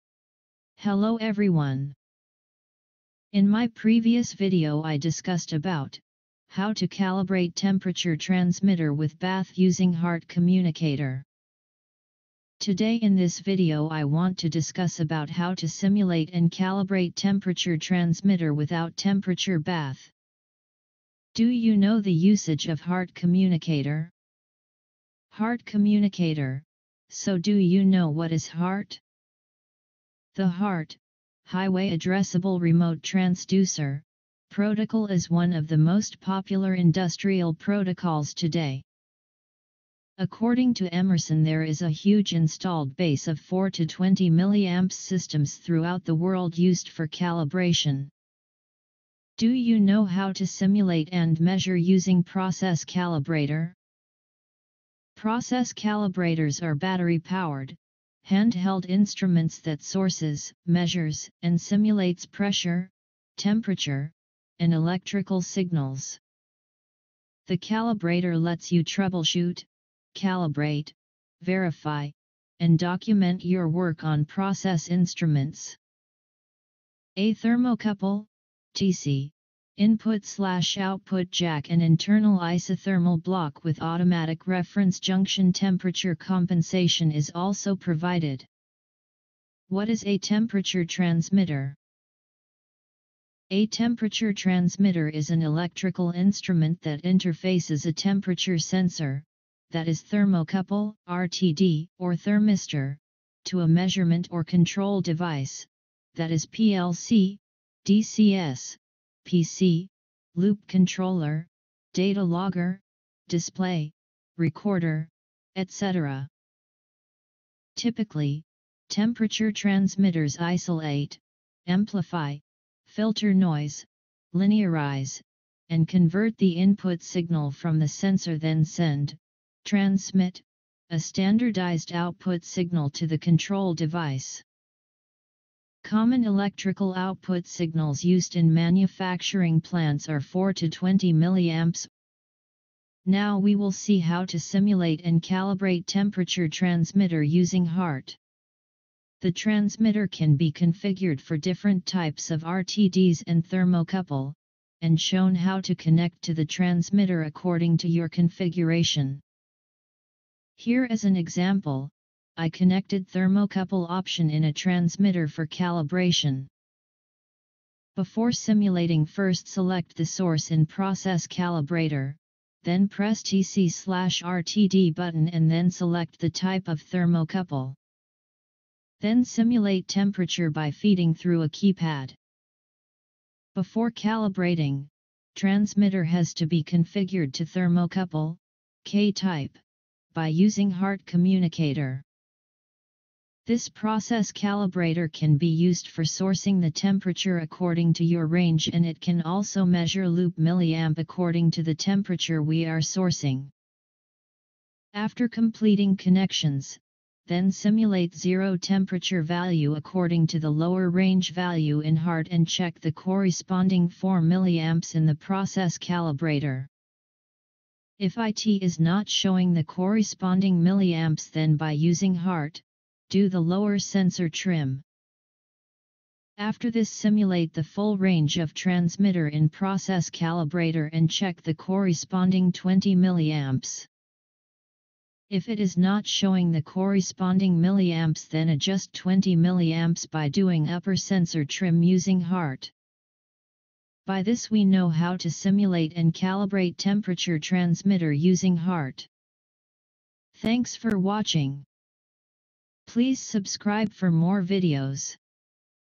How to simulate and calibrate temperature Transmitter without temperature bath using HART
how many know how to calibrate differential pressure (DP) transmitter using HART COMMUNICATOR?

do you know the usage of hart communicator?

Today i want to tell how to calibrate a differential pressure transmitter and instruments used for doing calibration.

HART Communicator: So do you know what is HART?
The HART(Highway Addressable Remote Transducer) protocal is one of the most popular industrial protocols today. According to emerson there is a huge installed base of 4 to 20 milliAmps systems throughout the world used for calibration

what is Loop Calibrator?
For process calibration professionals, loop calibrators are essential tools for working with 4 to 20 milliAmps current loops. Process control loop calibrators provide milliAmps sourcing, simulation and measurement, readouts in both milliAmps and % of span, 24 Volts loop supply.

What is Digital Pressure Calibrator means?

It is a portable,pressure indicator, This micro processor based instrument uses an internal transducer to measure and display pneumatic pressures applied to the connection port, This contains pneumatic hand pump,volume adjuster & vent valve

CALIBRATE DIFFERENTIAL PRESSURE TRANSMITTER using HART:

1. Static pressure test: Give equal pressure on both sides of the transmitter, Zero should not shift, If it is shifting carry out static alignment.

2.After completing static test then give Zero pressure to High Pressure side of transmitter and check the corresponding 4 milliAmps in the loop calibrator.

3.Low Pressure side should be vented to atmosphere.

4.IF IT IS NOT SHOWING THE CORRESPONDING milliAmps the using HART do the lower sensor trim.

5.After this apply full range of transmitter using Digital pressure Calibrator and check the corresponding 20 milliAmps .

If it is not showing the corresponding milliAmps the adjust 20 millliAmps by doing Upper sensor trim using HART.

this how we can calibrate differential pressure transmitter using HART

this how we will do calibration by hart communicator